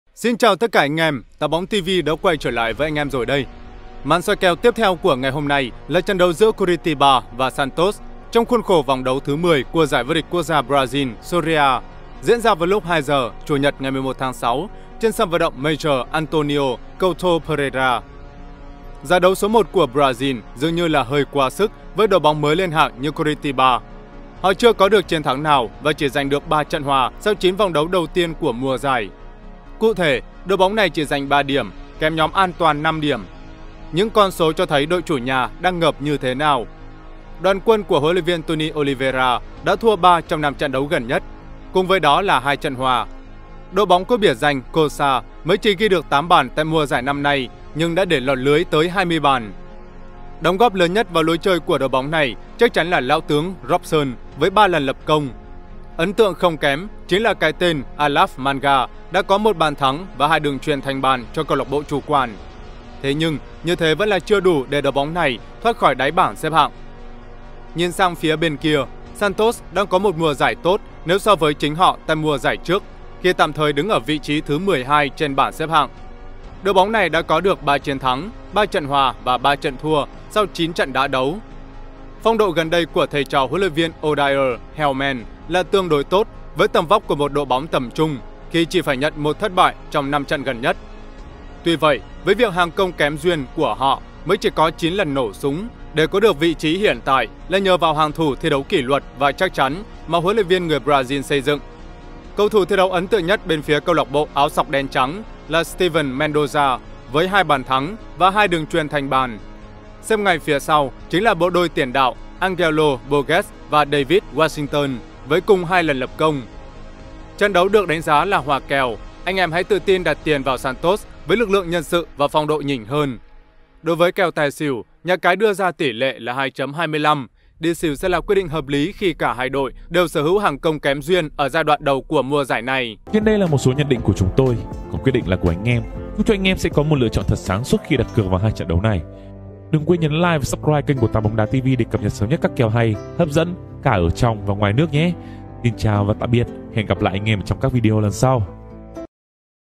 SOI KÈO, NHẬN ĐỊNH BÓNG ĐÁ | SERIE A BRAZIL | CORITIBA VS SANTOS
Xin chào tất cả anh em! 8bongtv đã quay trở lại với anh em rồi đây. Màn soi kèo tiếp theo của ngày hôm nay là trận đấu giữa Coritiba vs Santos, trong khuôn khổ vòng đấu thứ 10 của giải vô địch quốc gia Brazil Serie A, diễn ra vào lúc 02h00 CN ngày 11/06 trên sân vận động Major Antônio Couto Pereira.
KÊNH YOUTUBE CHÍNH THỨC CỦA [8BONGTV.COM]